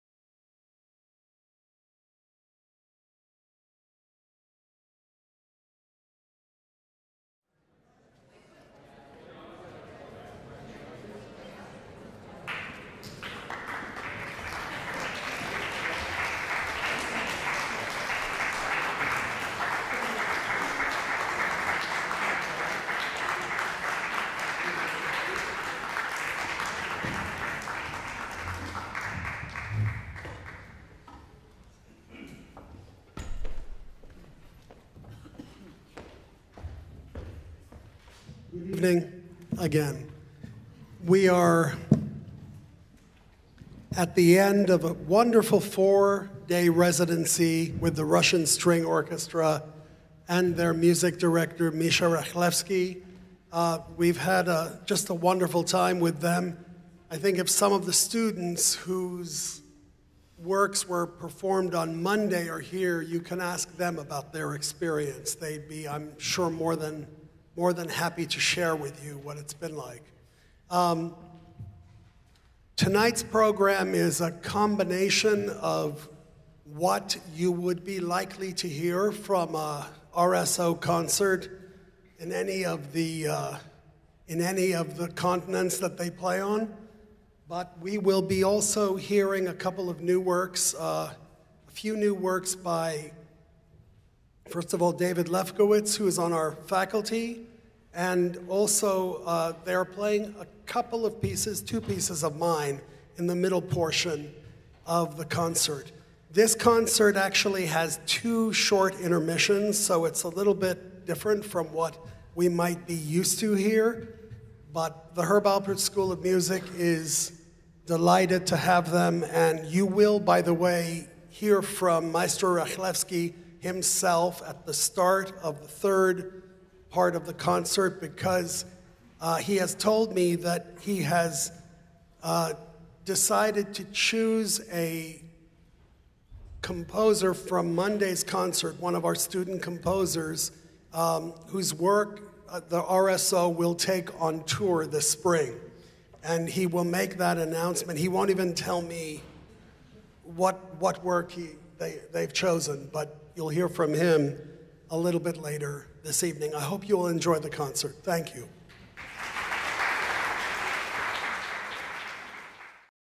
Richard Danielpour's introduction to the concert by Russian String Orchestra
Richard Danielpour's introduction to the concert by Russian String Orchestra (formerly Chamber Orchestra Kremlin), conducted by Misha Rachlevsky, on November 7, 2018, at the Schoenberg Hall at UCLA.